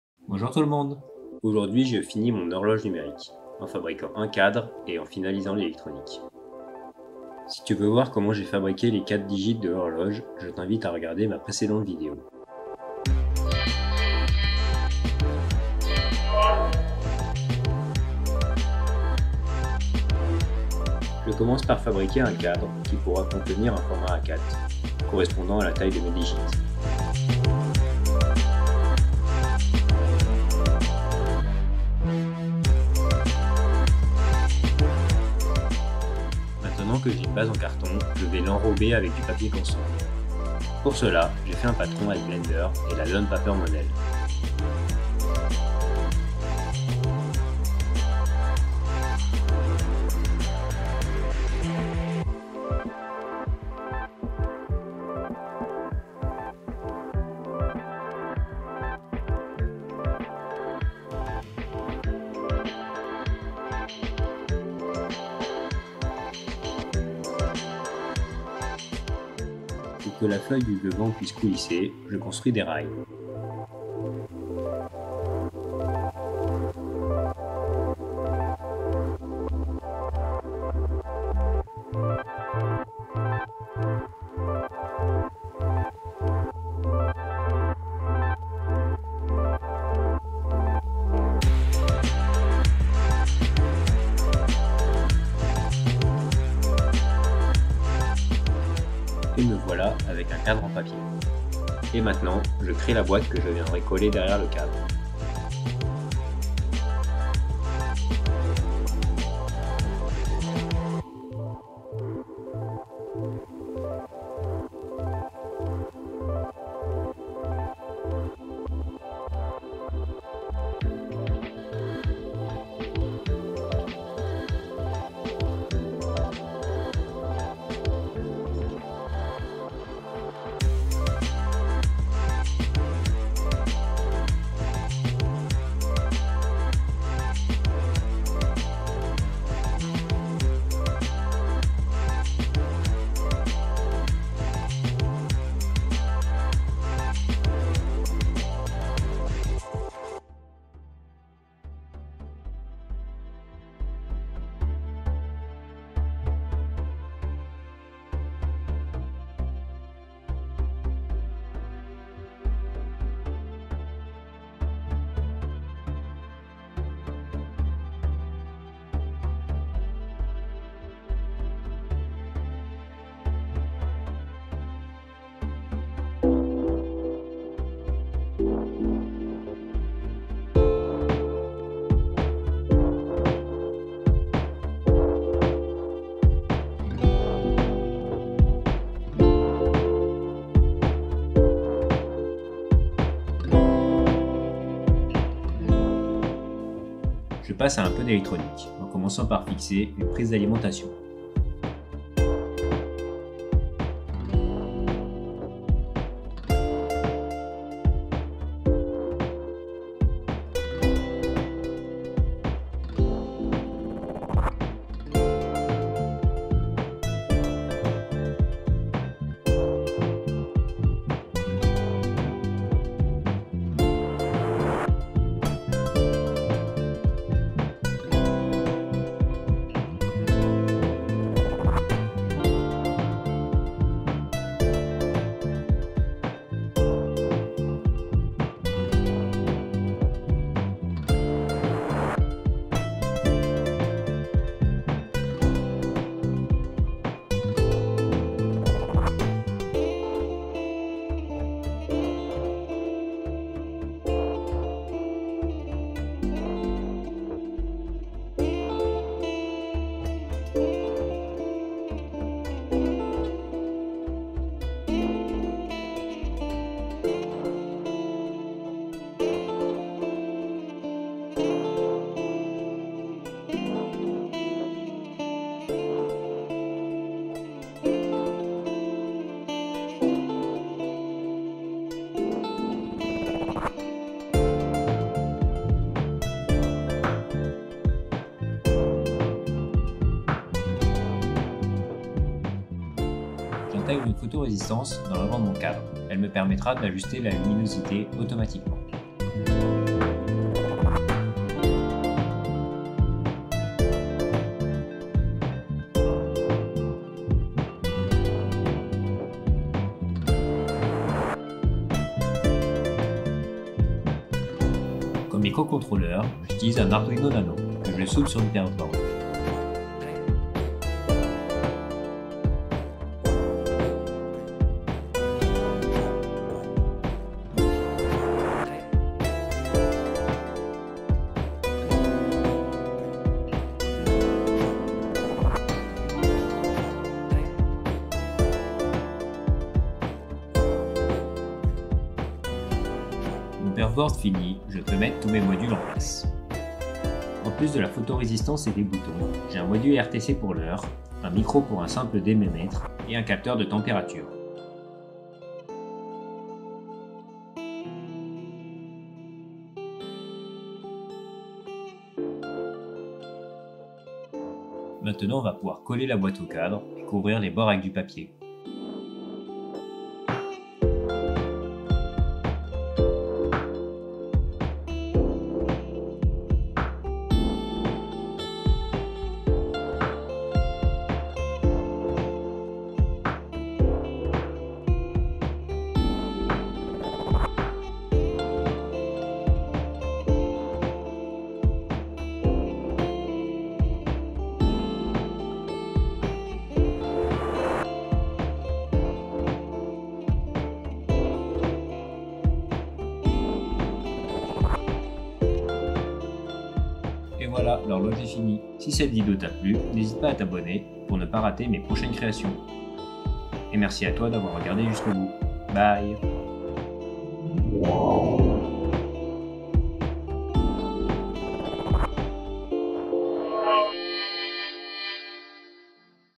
Horloge Digitale invisible, en papier ?
Aujourd'hui je finis l'horloge digitale en papier, je fabrique un cadre, une boîte et l'électronique.

Ce que j'utilise:
- Arduino Nano
- Module RTC Ds3231
- Microphone Electret Max4466
- Thermometre Digital Ds18b20
- Bouton Poussoir CMS

0:00 - Intro
0:18 - Fabrication du Cadre
1:40 - Fabrication de la Boite
2:43 - Placement des Digits
3:14 - Electronique
5:42 - Assemblage des module sur le Perfboard
6:02 - Finition
6:41 - Beauty Shot

@ Basile Rouault